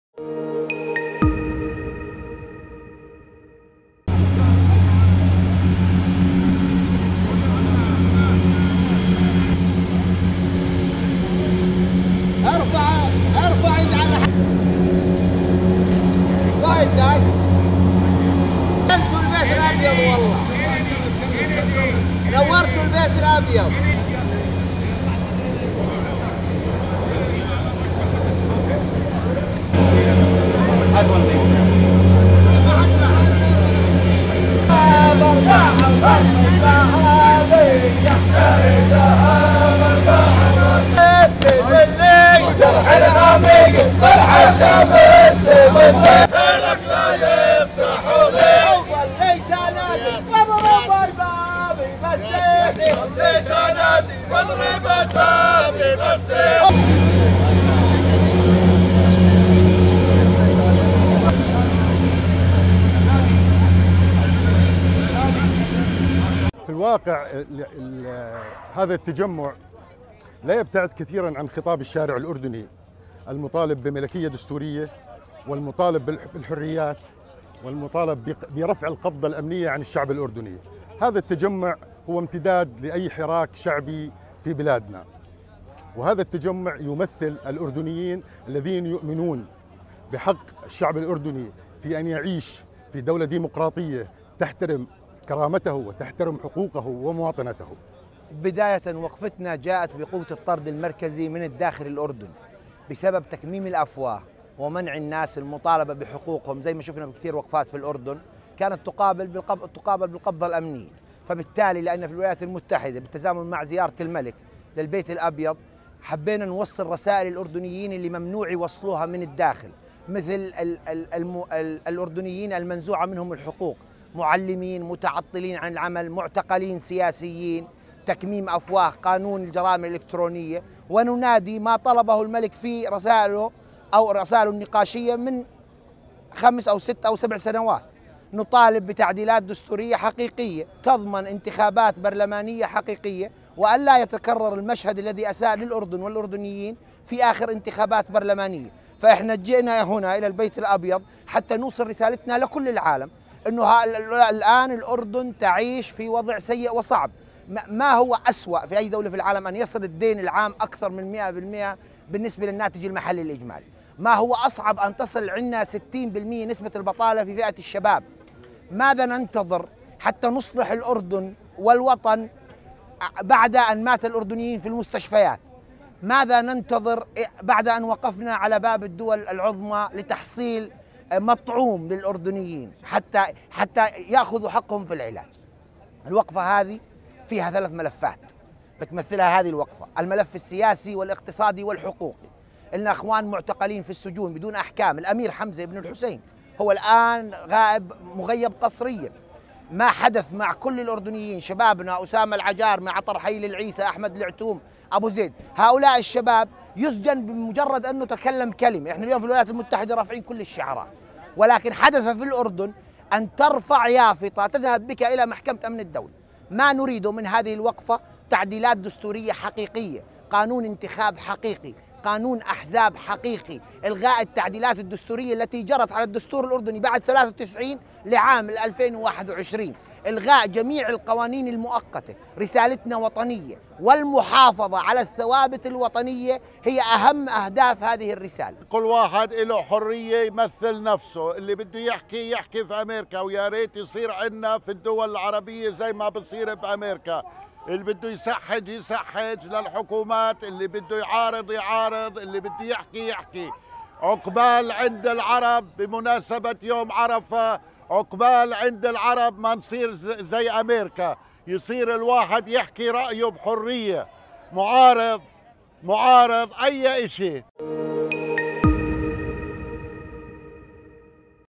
US- Jordanian Opposition Organized a Sit-in Near the White House
Following Jordan's King Abdullah II and US President Joe Biden's summit, a number of Jordanian opposition members gathered in front of the White House to clamor for political, economic, and rights reforms